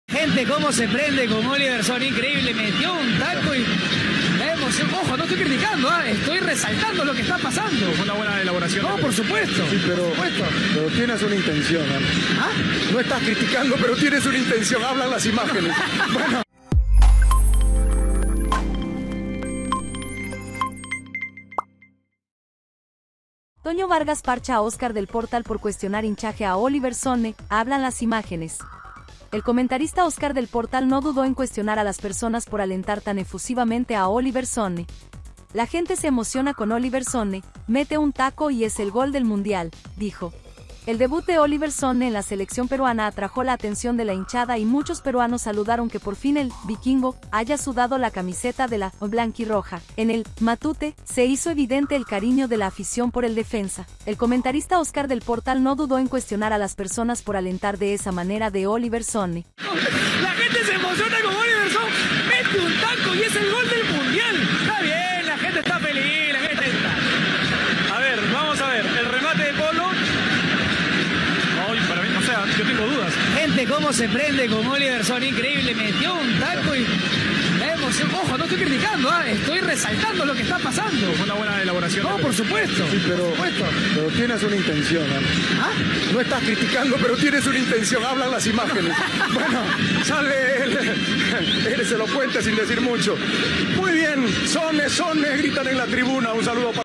Toño Vargas PARCHA en vivo a Óscar del Portal por cuestionar hinchaje a Oliver Sonne
El comentarista Óscar del Portal no dudó en cuestionar a las personas por alentar tan efusivamente a Oliver Sonne. “La gente se emociona con Oliver Sonne: mete un taco y es el gol del Mundial”, dijo .